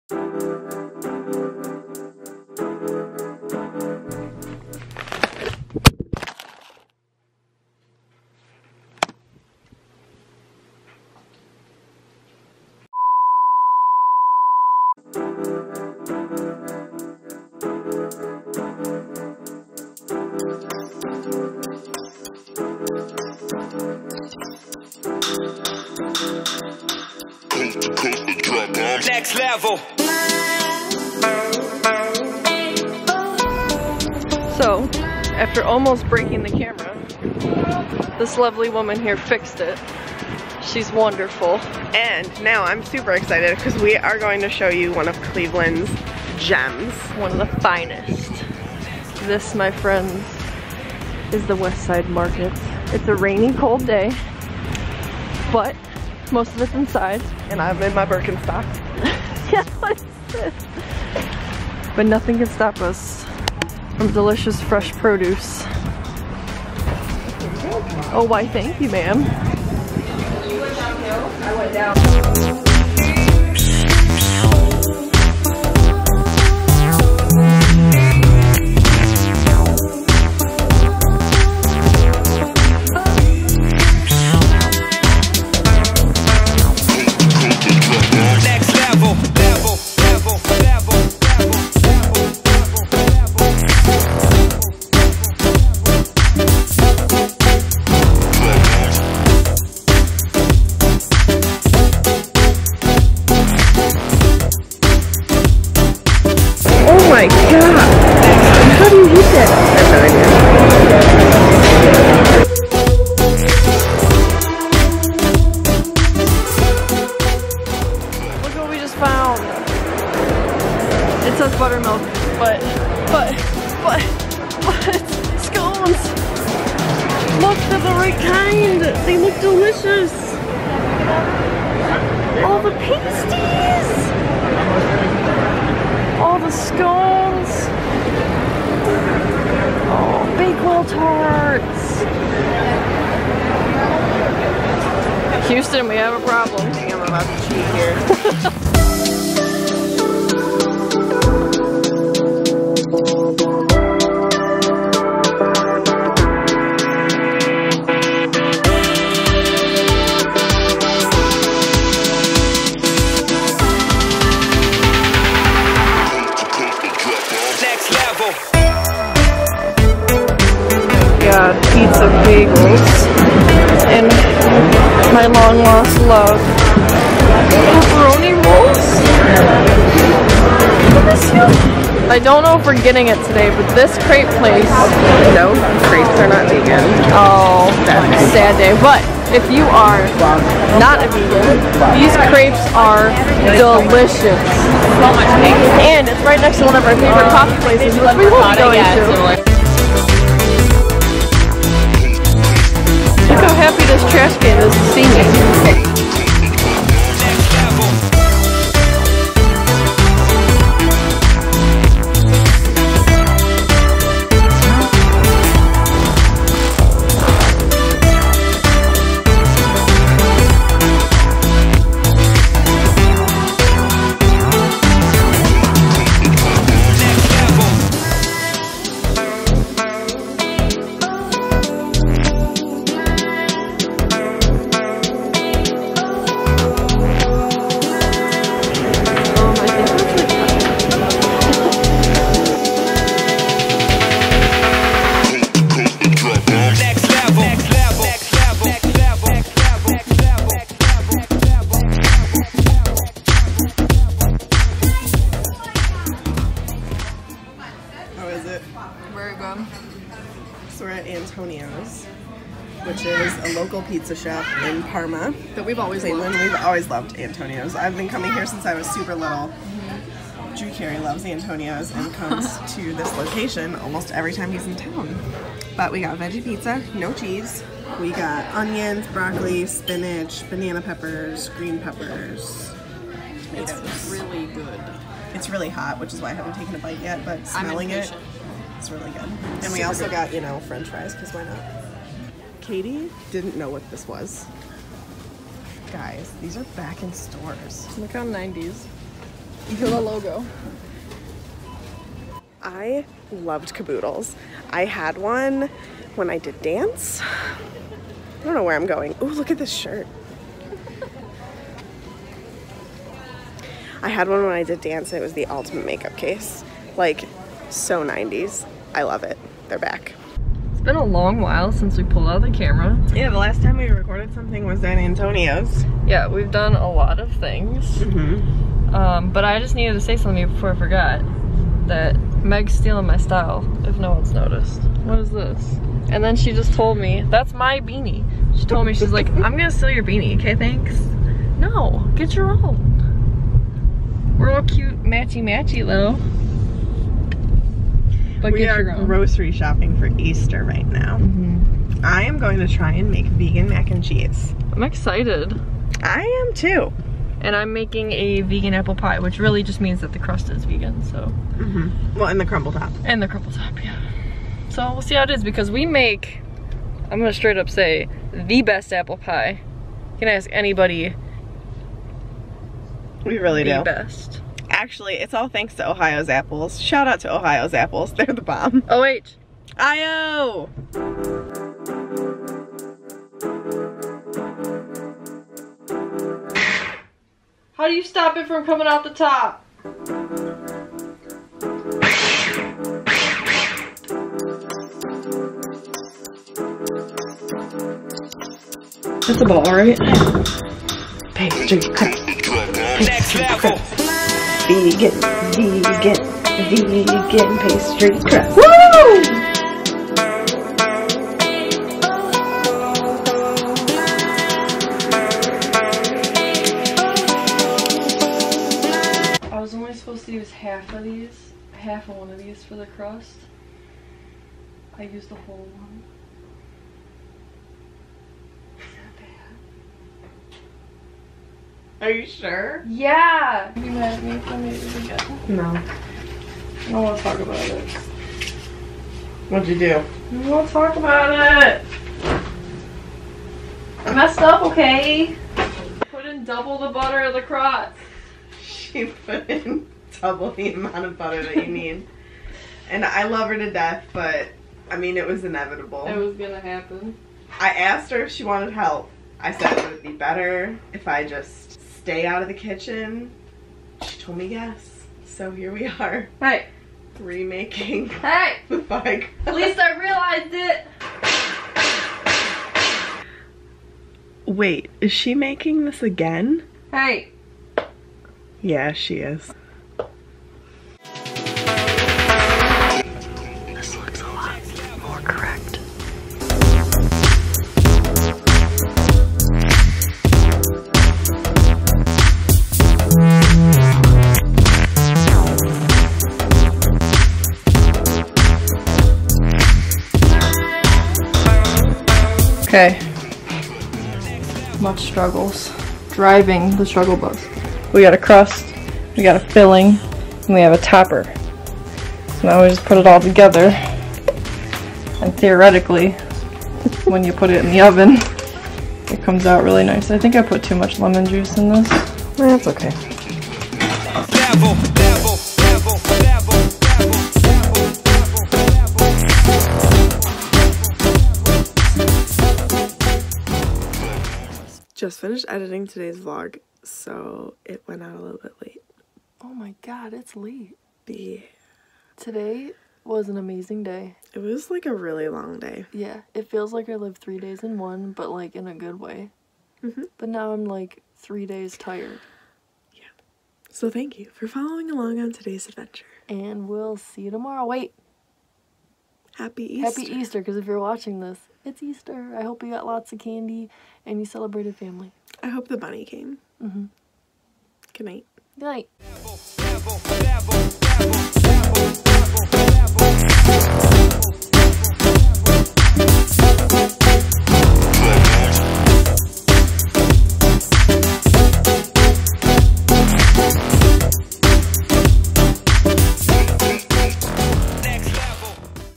Cleveland's West Side Market! | Veggie Pizza, and Apple Pie Fails
We take you to the iconic West Side Market! We try some veggie pizza from one of our favorite local pizza places and Katie tries, fails, tries again, and maybe succeeds at making a vegan apple pie?

Follow our podcast, Swish & Flick: An All Potter Podcast, on iTunes, Podbean, Google Play and more.